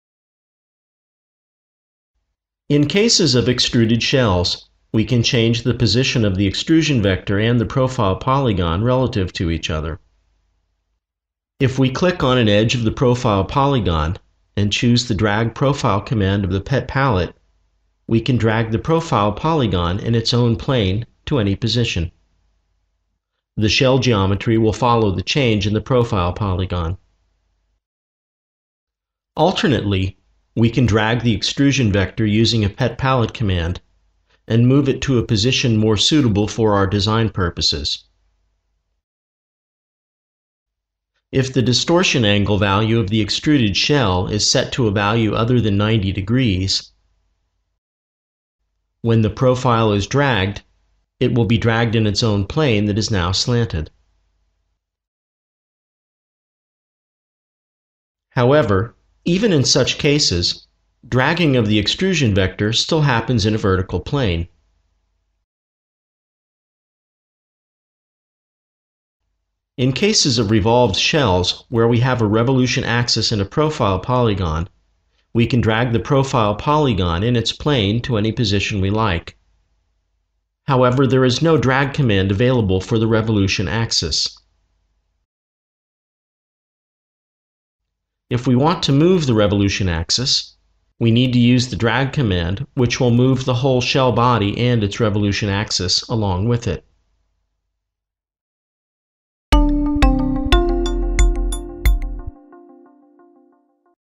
The ARCHICAD Shell Tool - Drag profile/drag extrusion vector
ArchiCAD 15 New Features - Shells
Drag profile/drag extrusion vector

In case of Extruded Shells we can change the position of the Extrusion Vector and the Profile Polygon relative to each other. If we click on an edge of the Profile Polygon and choose the Drag Profile command of the Pet Palette, we can drag the Profile Polygon in its own plane to any position. The Shell geometry will follow the change in the Profile Polygon.